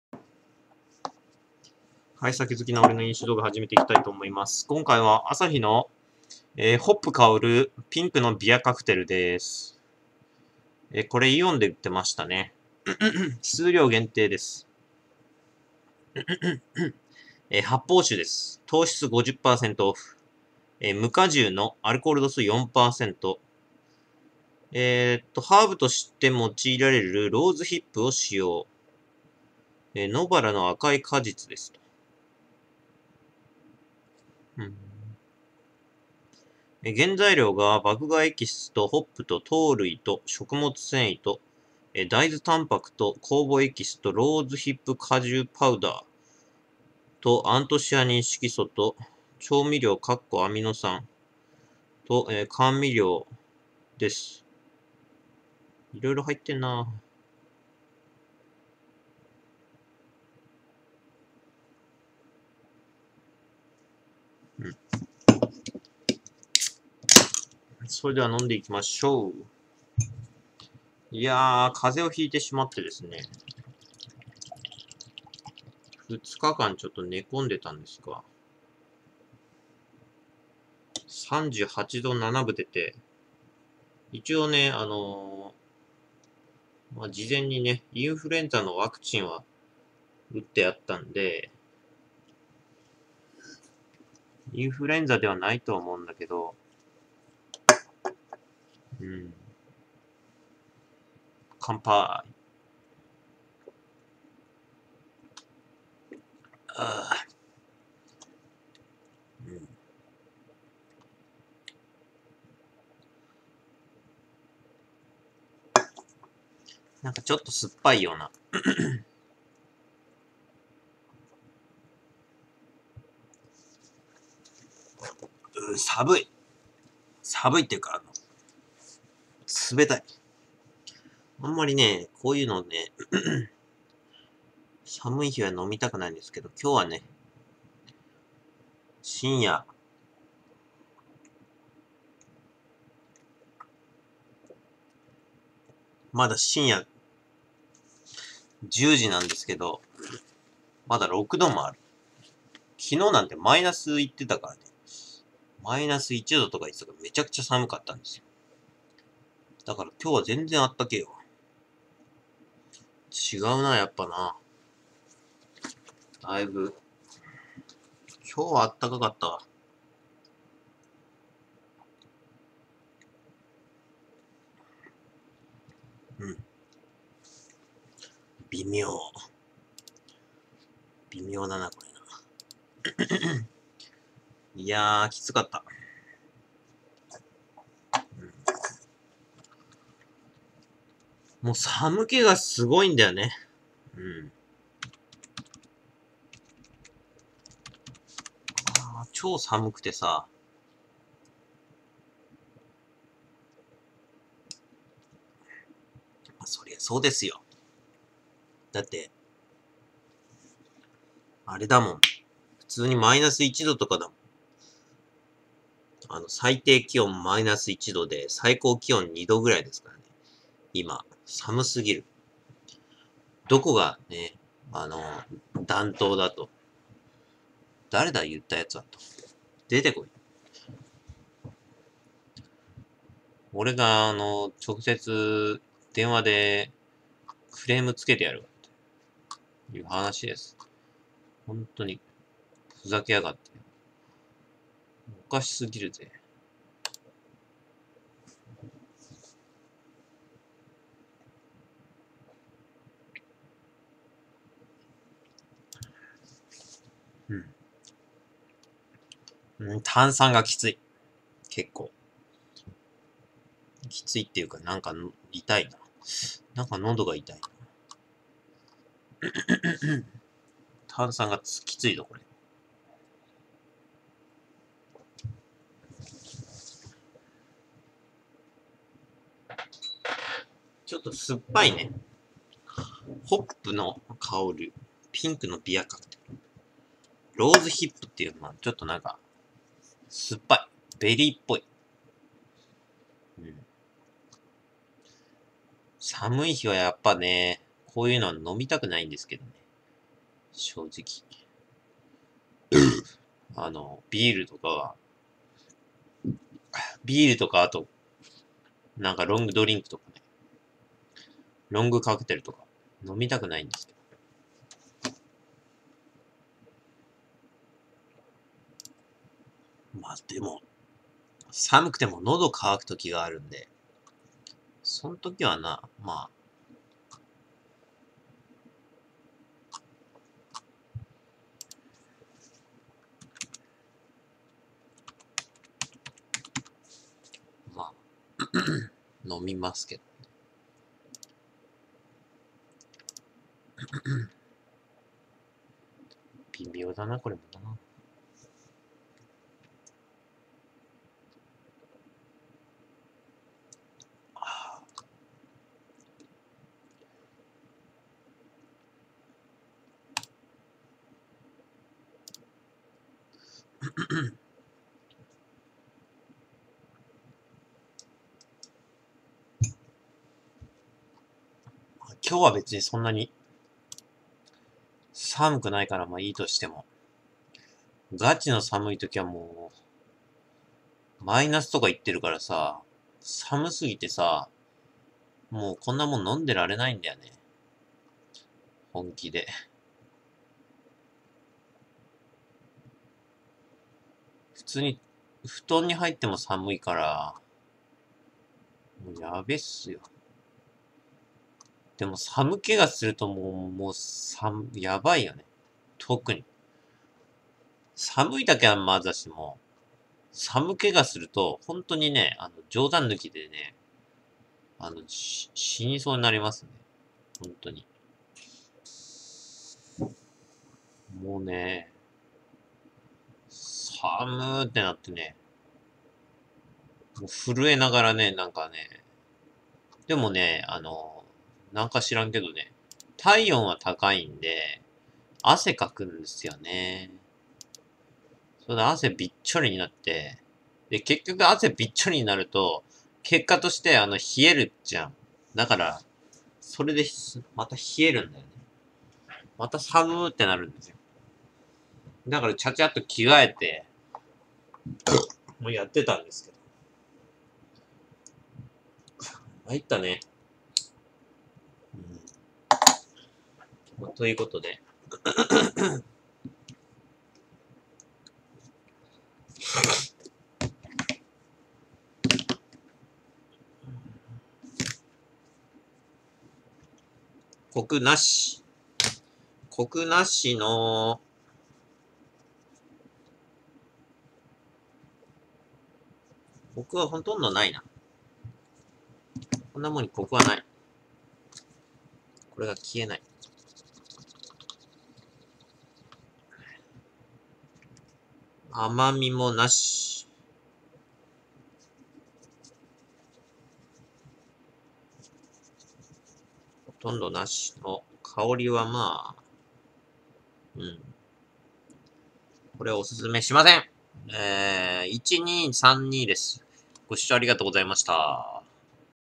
酒好きな俺の飲酒動画 part805 アサヒ ホップ香るピンクのビアカクテル 【発泡酒】
甘み：★★
香り：★★★
おススメ度：★★ 毎日の夜はニコニコ生放送にて生放送をしております。
気軽にLINEで友達になろう！LINE⇒ twitterやってます。